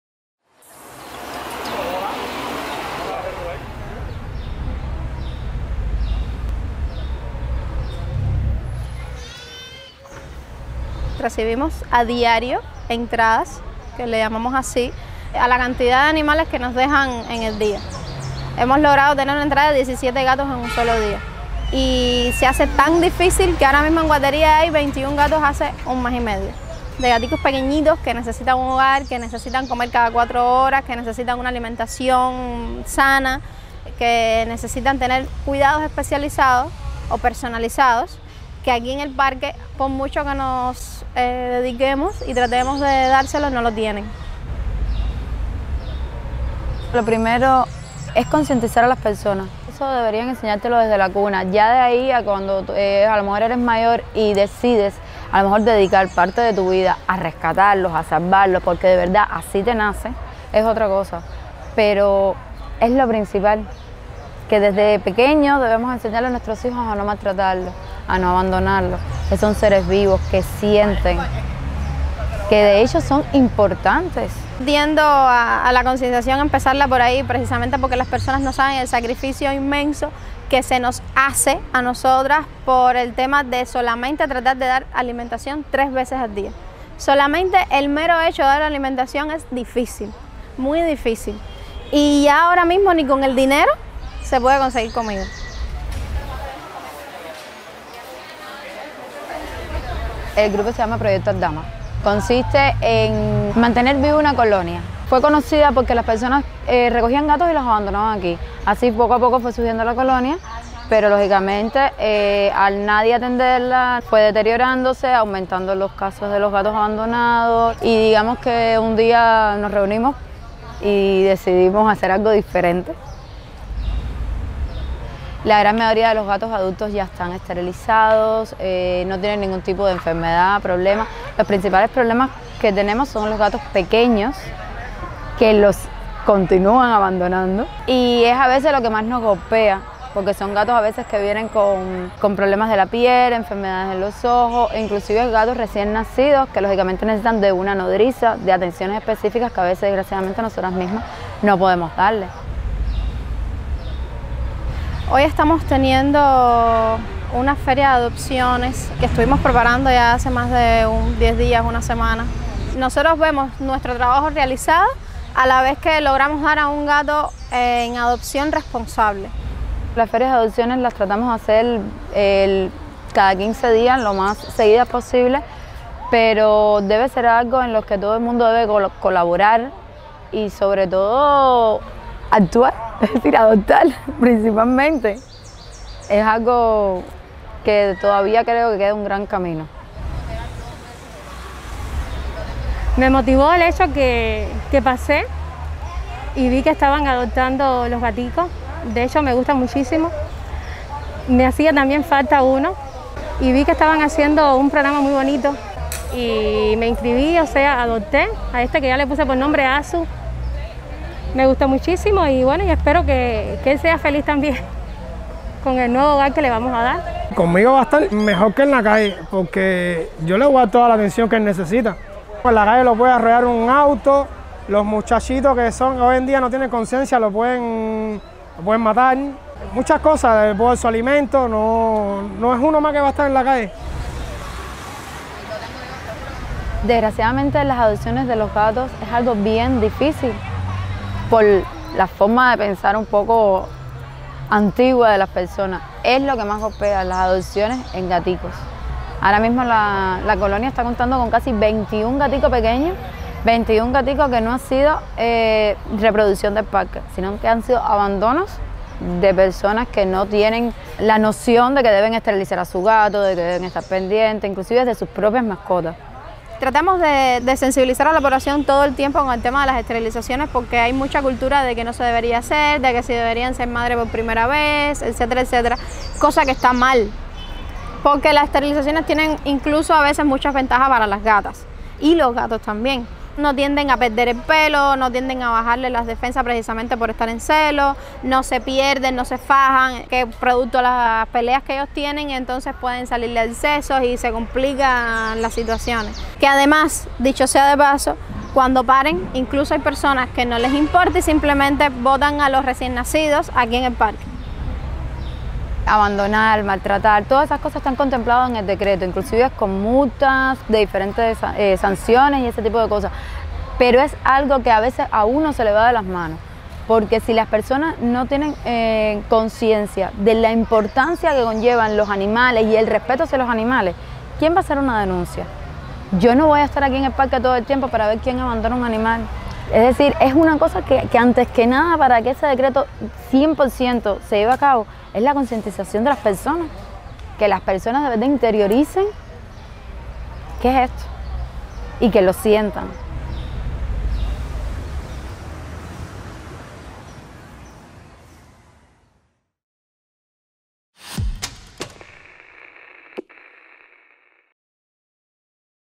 El parque de los gatos huérfanos
Un grupo de jóvenes cubanos creó Aldama, un proyecto para ayudar a la colonia de gatos ubicada en este lugar ante el paulatino aumento del abandono de felinos. Cada 15 días organizan una feria de adopción y concientización sobre cómo cuidar a estos animales.